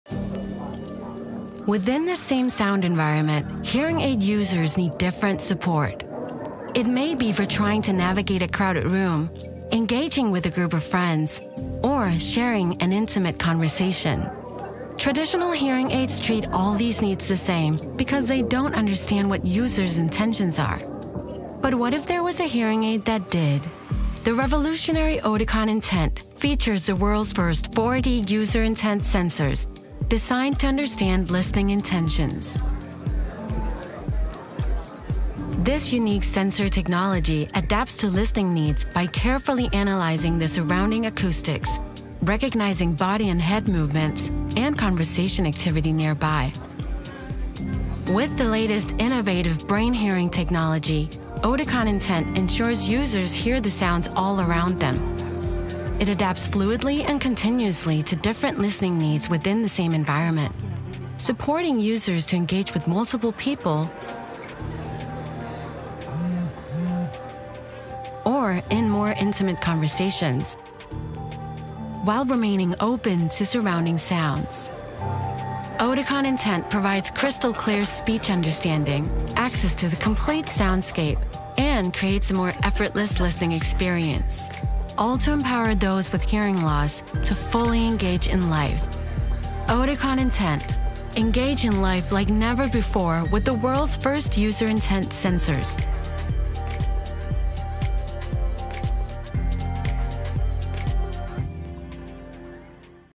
2024년 4D 모션센서를 탑재한 오티콘 인텐트 (Intent) 컨셉영상
안녕하세요. 오티콘보청기입니다.

토탈청각솔루션 기업 오티콘보청기에서  보청기 최초 4D 모션센서, 오티콘 신제품 인텐트(Intent) 프리런칭을 진행하였습니다!

오티콘 신제품 인텐트(Intent)는 새로운 플랫폼 시리우스(Sirius)가 탑재되어

10%향상된 초고음질로 청취
13% 정교해진 말소리 청취
10% 더 편안한 청취
15%말소리 이해력 증가가
가능합니다.

또한 4D 센서 기술을 탑재하여 보청기 착용자의 머리와 몸의 움직임을
미세하게  감지하여 360° 들려오는 소리의 세부사항을 놓치지 않게 해줍니다.

보다 자세한 사항은 아래링크에서 확인해 주시기 바랍니다.